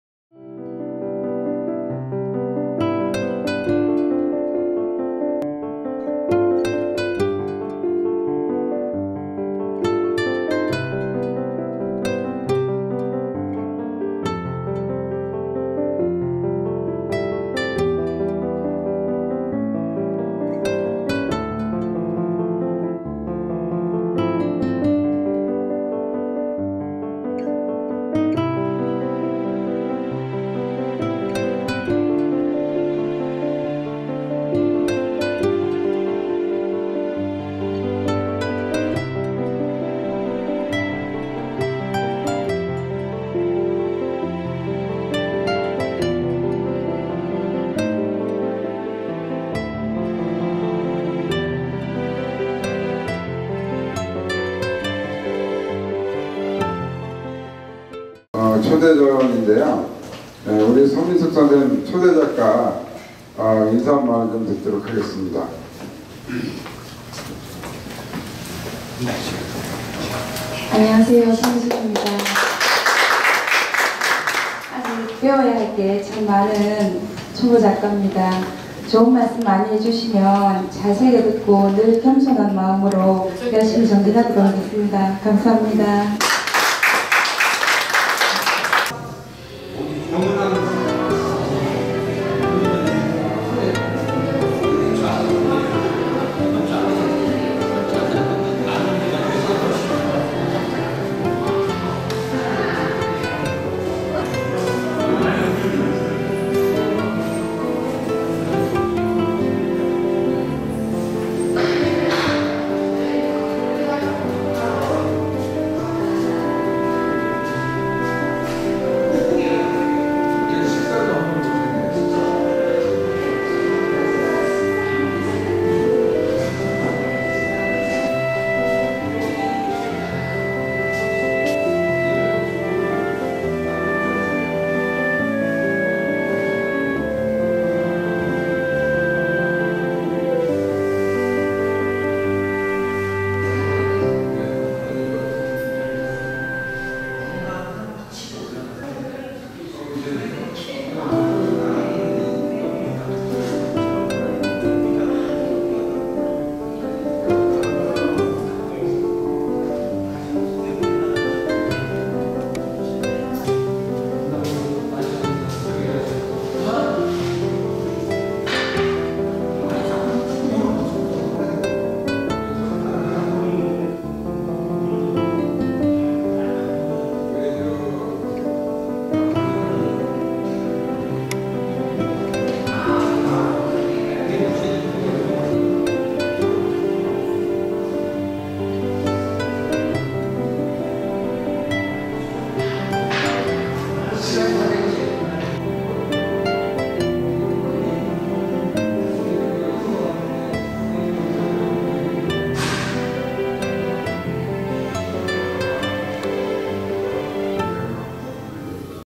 대상작가 서미숙 초대전
김재완 기자 = 서울 종로구 인사동 중심가에 위치한 인사아트프라자갤러리 4층 B관에서 제14회 대한민국수채화대전에서 영예의 대상을 수상한 서미숙 작가 초대전이 8월 1일부터 7일까지 개최되었다.